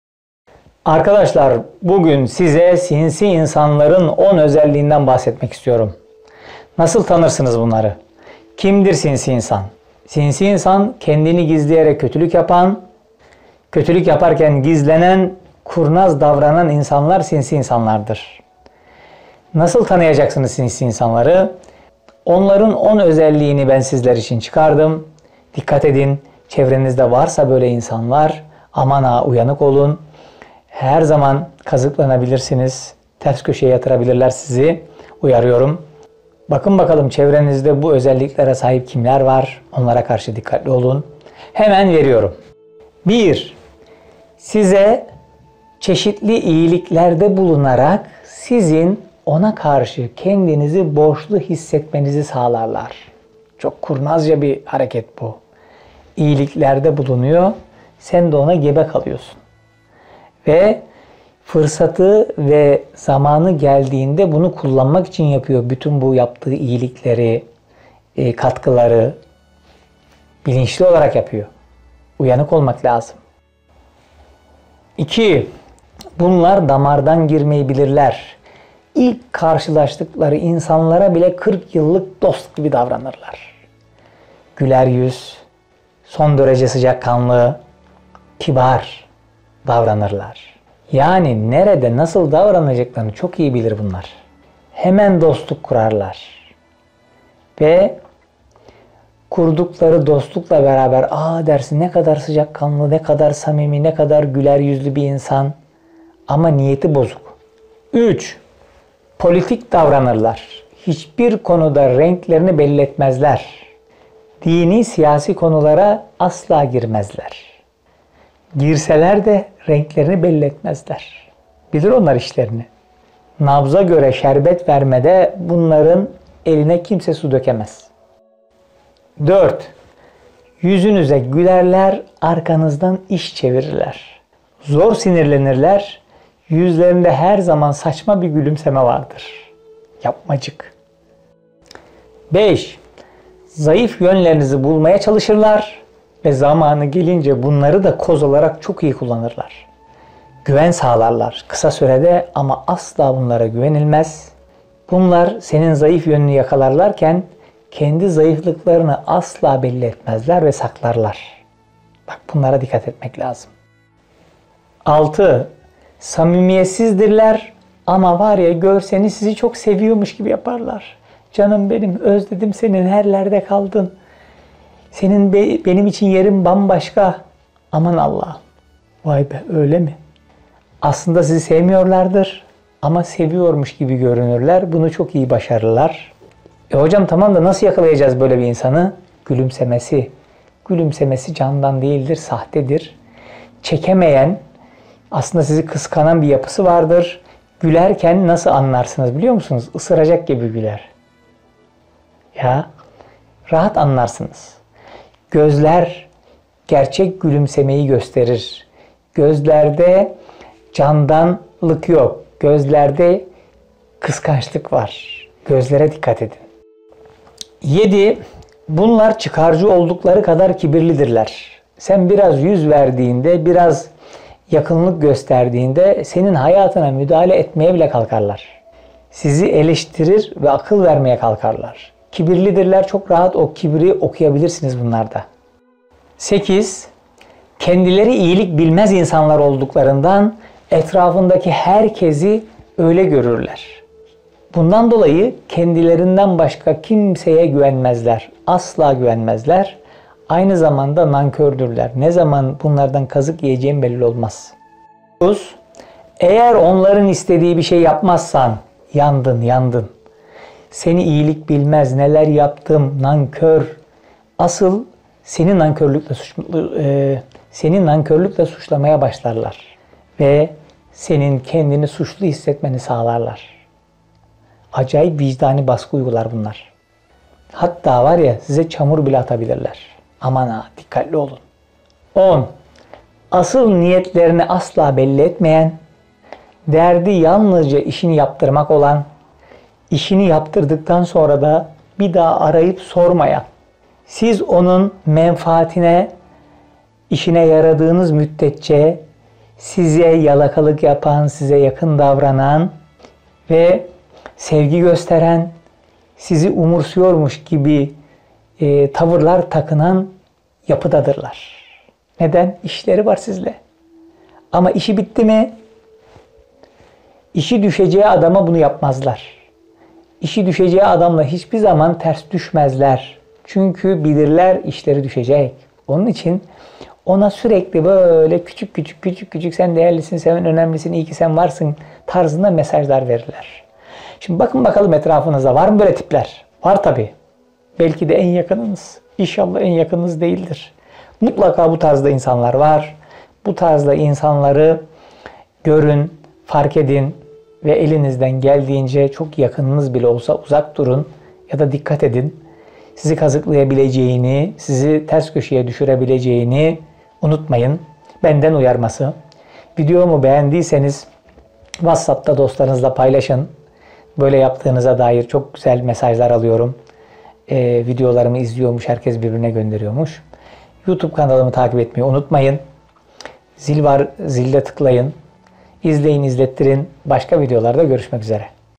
SİNSİ İNSANLARIN 10 ÖZELLİĞİ | Canten Kaya
Sinsi insan kimdir? Sinsi İnsanların en belirgin özellikleri nelerdir?
Sinsi insanları nasıl tanırsınız?